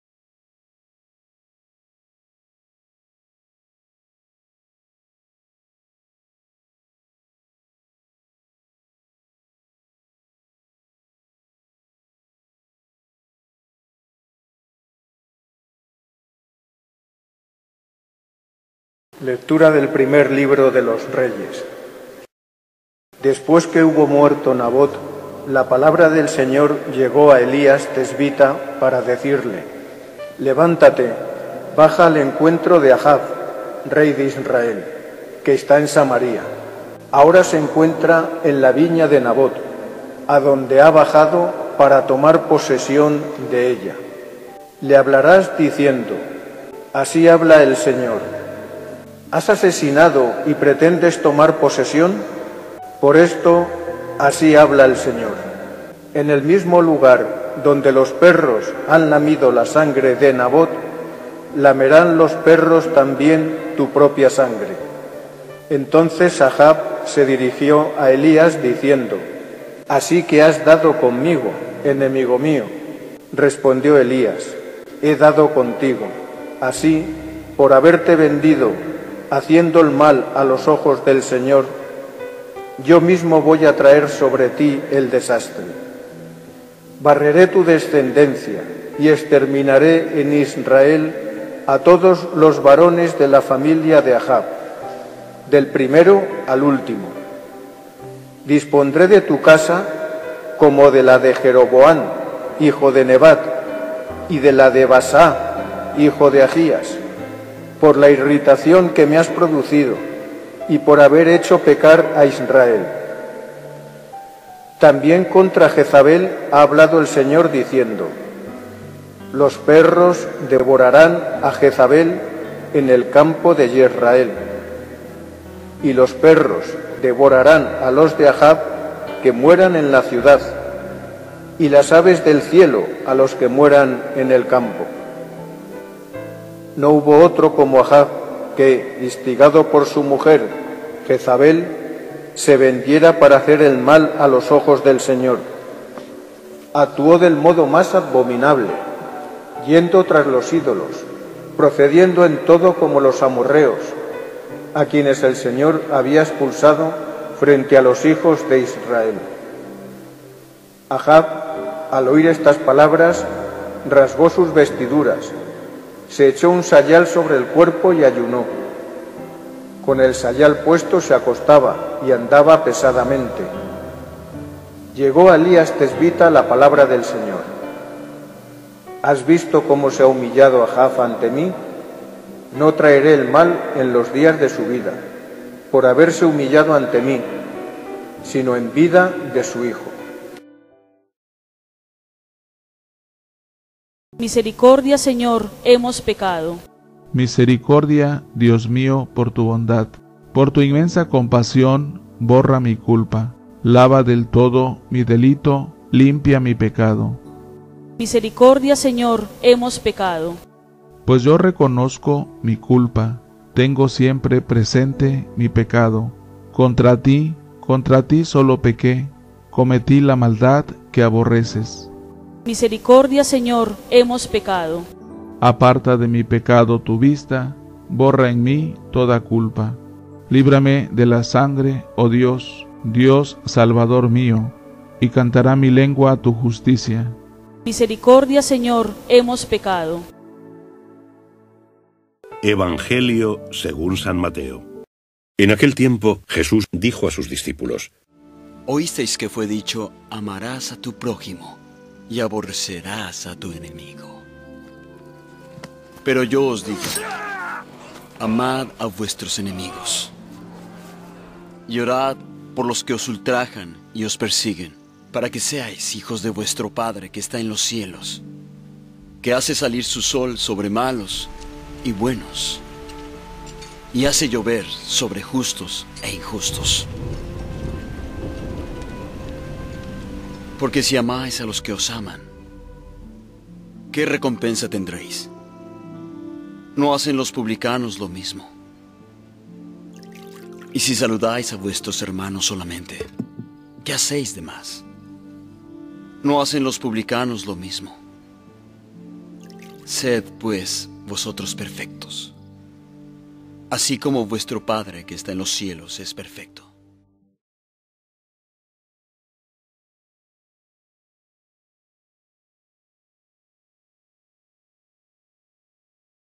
Evangelio Martes 16 de Junio de 2020 Mateo 5,43-48
Audio Video Lecturas de la Santa Eucaristía día Martes 16 de Junio de 2020 MARTES XI SEMANA TIEMPO ORDINARIO CICLO A Primera Lectura: Del primer libro de los Reyes 1R 21, 17-29 Salmo 50,3-4.5-6a.11.16 R/. Misericordia, Señor: hemos pecado Del santo Evangelio según San Mateo 5, 43-48 PRIMERA LECTURA
Has hecho pecar a Israel
Lectura del primer libro de los Reyes 21, 17-29
Después que hubo muerto Nabot, la palabra del Señor llegó a a Elías tesbita para decirle:
-«Levántate, baja al encuentro de Ajab, rey de Israel, que está en Samaria. Ahora se encuentra en la viña de Nabot, adonde ha bajado para tomar posesión de ella. Le hablarás diciendo: “Así habla el Señor: ‘¿Has asesinado, y pretendes tomar posesión?’ Por esto, así habla el Señor: ‘En el mismo lugar donde los perros han lamido la sangre de Nabot, lamerán los perros también tu propia sangre’”»
Entonces Ajab se dirigió a Elías diciendo:
-«Así que has dado conmigo, enemigo mío»
Respondió Elías:
-«He dado contigo. Así, por haberte vendido, haciendo el mal a los ojos del Señor, yo mismo voy a traer sobre ti el desastre. Barreré tu descendencia y exterminaré en Israel a todos los varones de la familia de Ajab, del primero al último. Dispondré de tu casa como la de de Jeroboán, hijo de Nebat, y de la de Baasá, hijo de Ajías, por la irritación que me has producido y por haber hecho pecar a Israel. También contra Jezabel ha hablado el Señor diciendo: “Los perros devorarán a Jezabel en el campo de Yezrael” , y lo perros devorarán a los de Ajab que mueran en la ciudad y las aves del cielo a los que mueran en el campo».
No hubo otro como Ajab que, instigado por su mujer Jezabel, se vendiera para hacer el mal a los ojos del Señor. Actuó del modo más abominable, yendo tras los ídolos, procediendo en todo como los amorreos a quienes el Señor había expulsado frente a los hijos de Israel.
Ajab, al oír estas palabras, rasgó sus vestiduras, se echó un sayal sobre el cuerpo y ayunó. Con el sayal puesto se acostaba y andaba pesadamente.
Llegó a Elías tesbita la palabra del Señor:
-«¿Has visto cómo se ha humillado Ajab ante mí? No traeré el mal en los días de su vida, por haberse humillado ante mí, sino en vida de su hijo».
Salmo 50, 3-4. 5-6a. 11 y 16
R/. Misericordia, Señor: hemos pecado.
Misericordia, Dios mío, por tu bondad,
por tu inmensa compasión borra mi culpa;
lava del todo mi delito,
limpia mi pecado. R/.
Pues yo reconozco mi culpa,
tengo siempre presente mi pecado:
contra ti, contra ti solo pequé,
cometí la maldad en tu presencia. R/.
Aparta de mi pecado tu vista,
borra en mí toda culpa.
Líbrame de la sangre, oh Dios,
Dios, Salvador mío,
y cantará mi lengua tu justicia. R/.
EVANGELIO
Amad a vuestros enemigos
En aquel tiempo, dijo Jesús a sus discípulos:
-«Habéis oído que se dijo: “Amarás a tu prójimo y aborrecerás a tu enemigo”. Pero yo os digo: amad a vuestros enemigos y rezad por los que os persiguen, para que seáis hijos de vuestro Padre celestial, que hace salir su sol sobre malos y buenos, y manda la lluvia a justos e injustos.
Porque, si amáis a los que os aman, ¿qué premio tendréis? ¿No hacen lo mismo también los publicanos? Y si saludáis sólo a vuestros hermanos, ¿qué hacéis de extraordinario? ¿No hacen lo mismo también los gentiles? Por tanto, sed perfectos, como vuestro Padre celestial es perfecto».